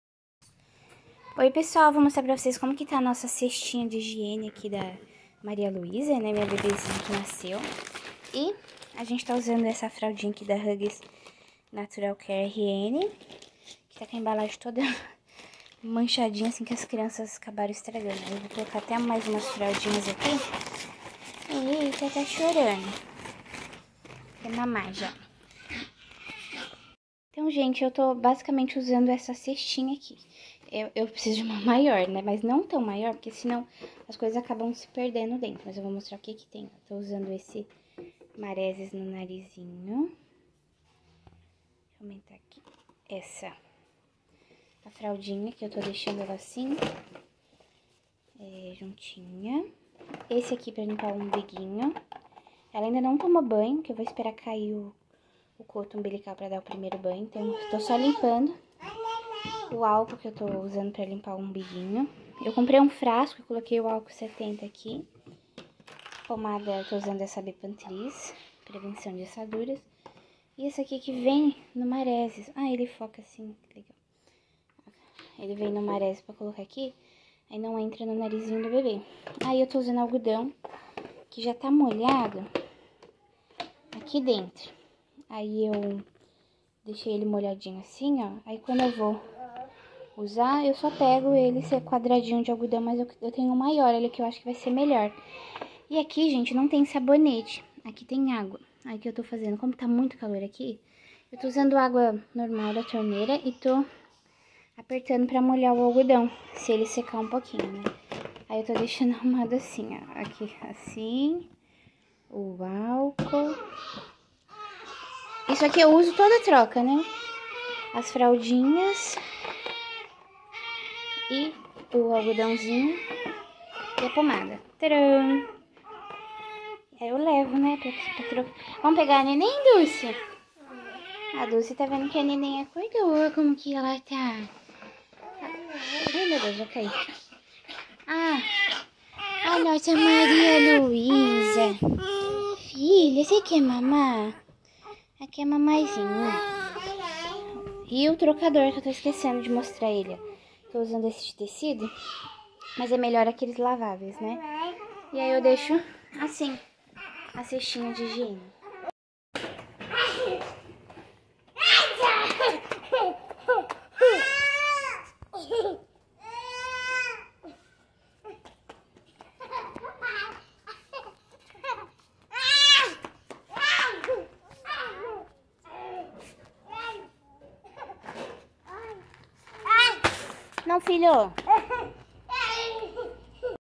KIT PARA TROCA DE FRALDA DA RN / Maria Luísa chegou 💕💕
Conheça a Comunidade Rotina Amável 🌺Toque AQUI: luaraspigotti.kpages.online/rotina-amavel

🌺 No vídeo de hoje mostro o kit que usamos para as trocas de fraldas na primeira semana com a Maria Luísa em casa 💕

Me siga no IG @luaspigotti